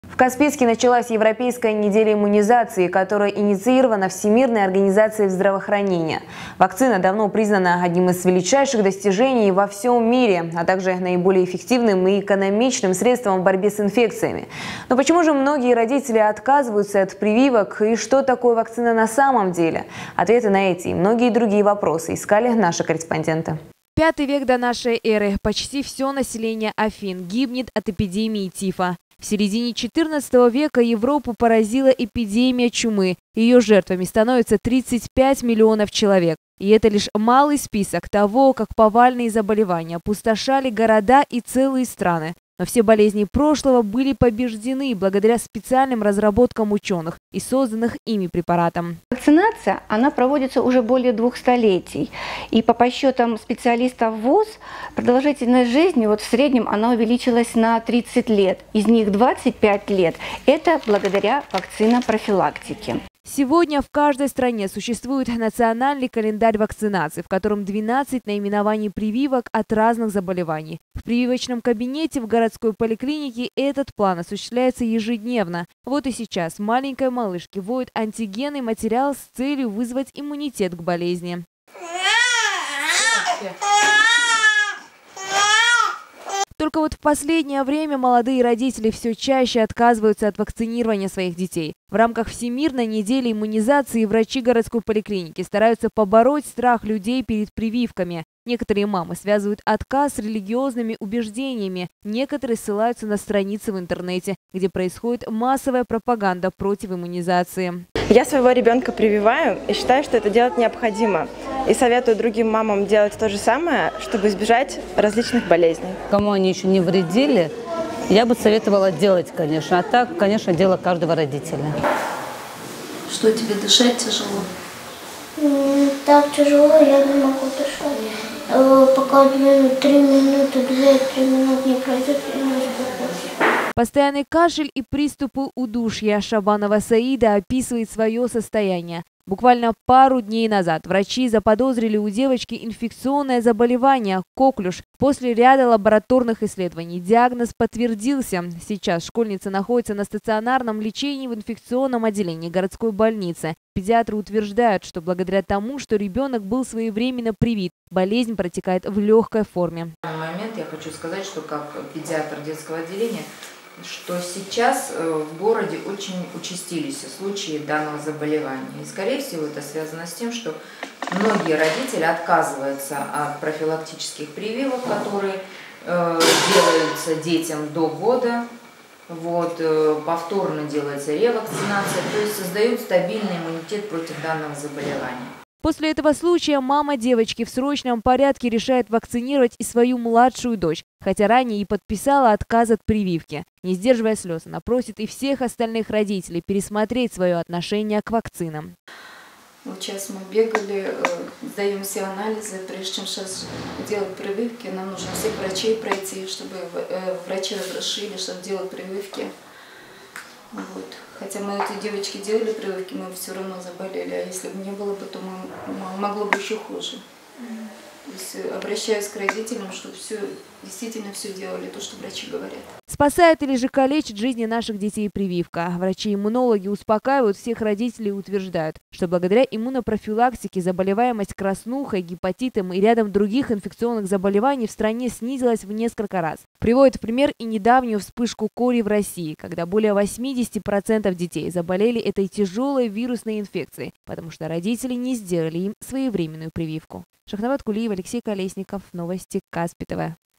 Почему многие родители отказываются от вакцинации своих детей?
Стоит ли делать прививки и почему многие родители отказываются от вакцинации своих детей?

В Каспийске началась Европейская неделя иммунизации, которая инициировала всемирная организация здравоохранения. Вакцина давно признана одним из величайших достижений во всем мире, а также наиболее эффективным и экономичным средством в борьбе с инфекциями. Но почему многие родители отказываются от прививок и что такое на самом деле вакцина? Ответы на эти и многие другие вопросы искали наши корреспонденты.

5 век до нашей эры, почти все население Афин гибнет от эпидемии тифа. В середине 14 века Европу поразила эпидемия чумы, ее жертвами становятся 35 миллионов человек. И это лишь малый список того, как повальные заболевания опустошали города и целые страны. Но все болезни прошлого были побеждены благодаря специальным разработкам ученых и  созданных ими препаратам.

Сегодня в каждой стране  существует  национальный календарь вакцинации, в котором 12 наименований прививок от разных заболеваний. В прививочном кабинете в городской поликлинике этот план осуществляется ежедневно. Вот и сейчас маленькой малышке вводят антигенный  материал с целью вызвать иммунитет к болезни.

Только вот в последнее время молодые родители, все чаще отказываются от вакцинирования своих детей. В рамках всемирной недели иммунизации врачи городской поликлиники стараются побороть страх людей перед прививками. Некоторые мамы связывают отказ с религиозными убеждениями, некоторые ссылаются на страницы в интернете, где происходит массовая пропаганда против иммунизации.

Постоянный кашель и приступы удушья, 12летняя  Шабанова Саида описывает свое состояние. Буквально пару дней назад, врачи заподозрили у девочки инфекционное заболевание - коклюш. После ряда лабораторных исследований диагноз подтвердился. Сейчас школьница находиться на стационарном лечении в инфекционном отделении городской больницы.  Педиатры утверждают, что благодаря тому, что ребенок был своевременно привит, болезнь протекает в легкой форме.

Шахнабат Кулиева, Алексей Колесников Новости КТВ.